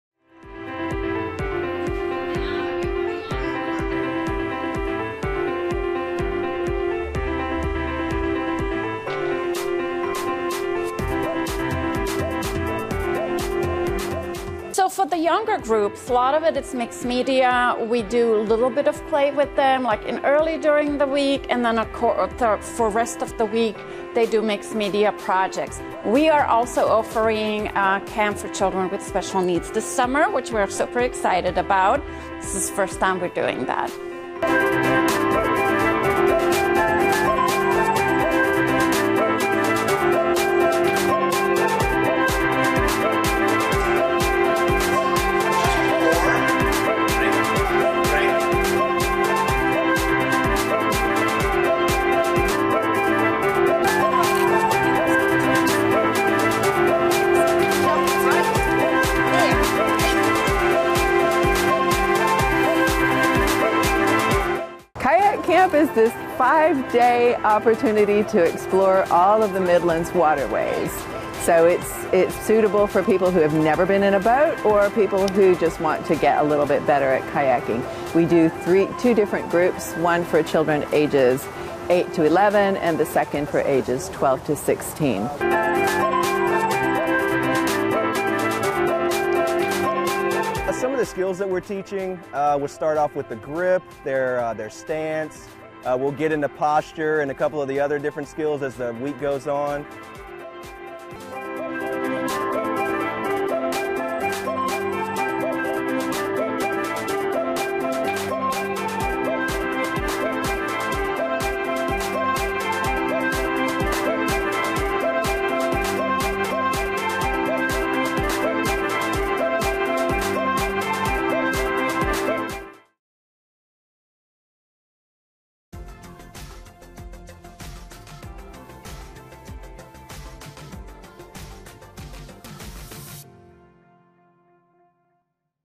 2023 City of Columbia Summer Camps
2023 City of Columbia Summer Camps  highlighting a variety of camps offered for children throughout the summer and upcoming camps.